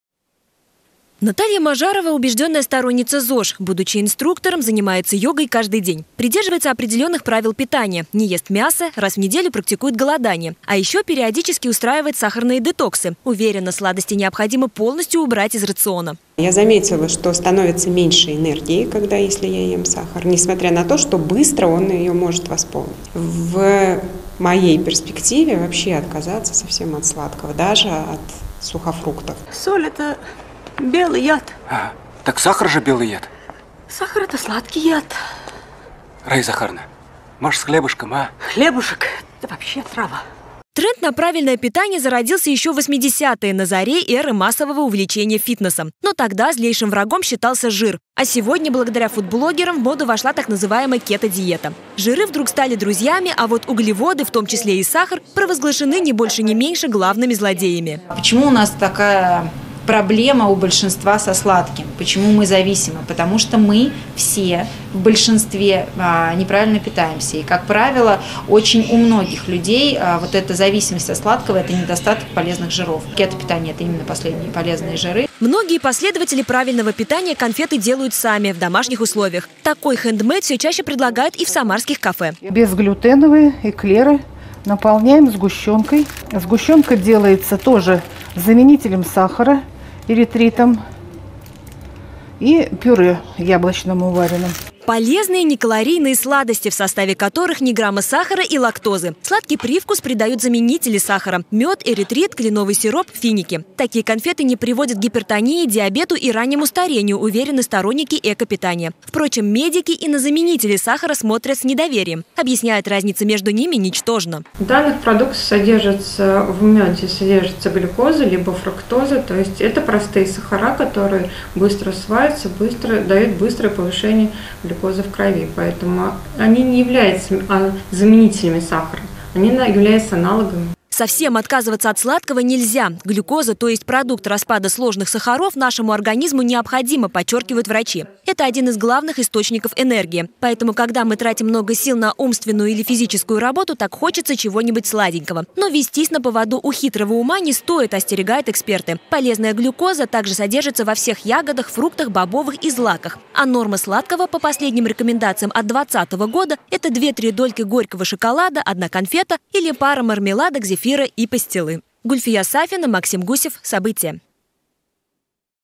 Как исключить из рациона сахар и при этом не навредить здоровью
Несладкая жизнь. По статистике россияне стали реже покупать сахар. На фоне активной агитации диетологов в стране растет число сторонников правильного питания.  Действительно ли вредно есть сахар и чем его можно заменить, расскажет Гульфия Сафина.